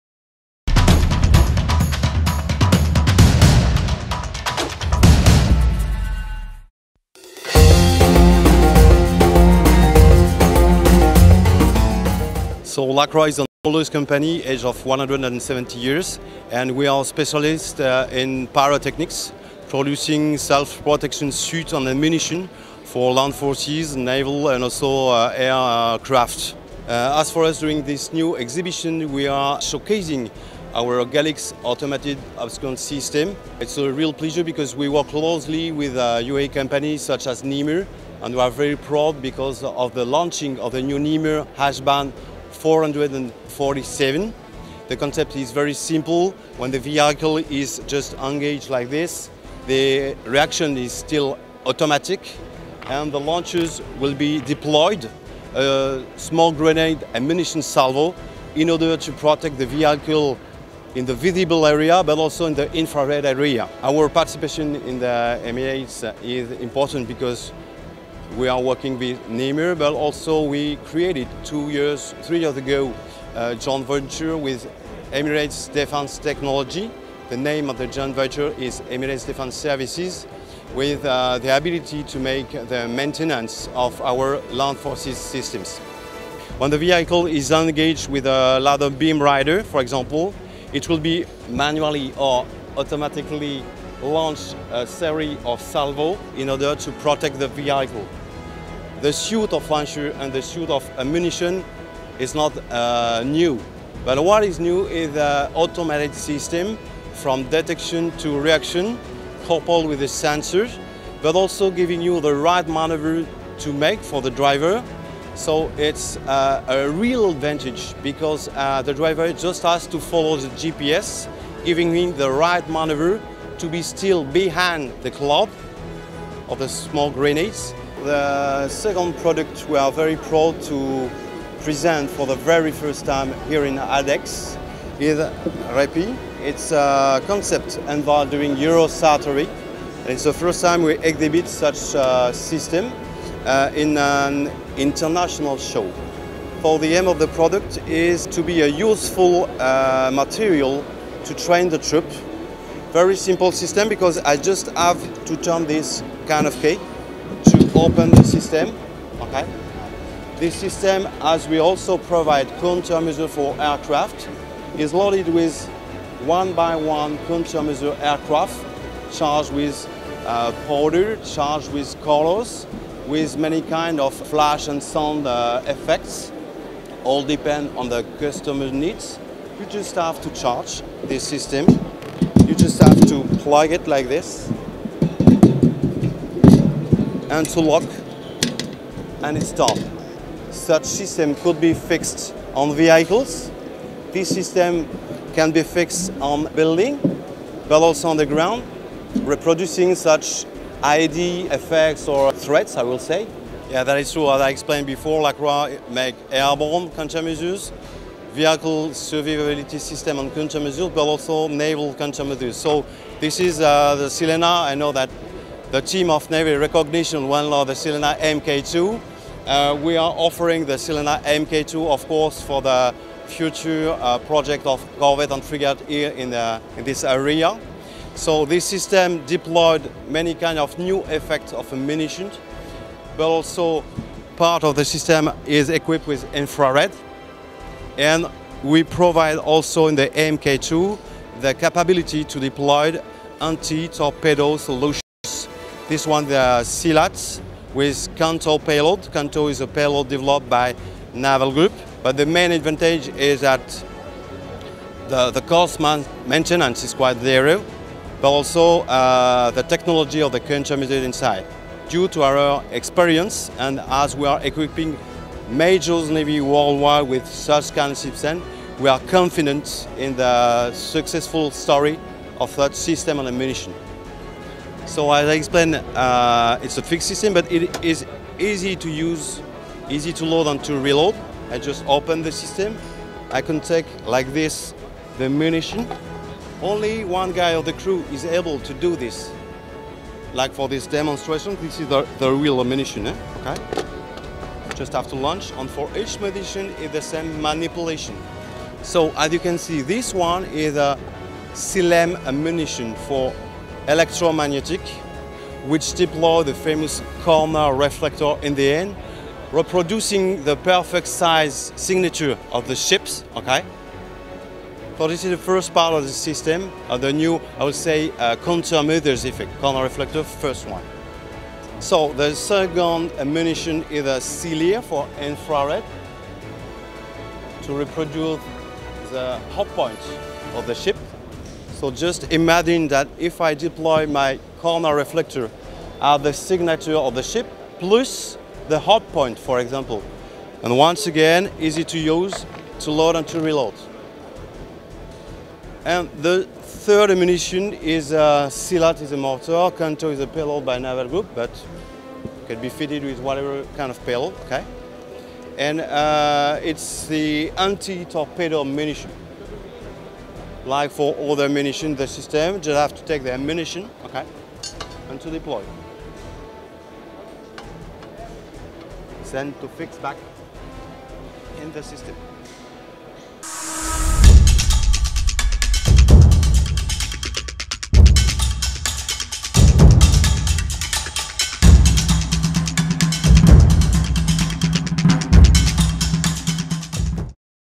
IDEX 2019 Lacroix from France world leader in defense countermeasures and pyrotechnic systems
At IDEX 2019, the French Company Lacroix showcases its Galix AOS (Automated Obscuration System), the Sylena MK2 decoy launching system, and its new realistic training system (REPY).

Read full news about Lacroix at IDEX 2019 to this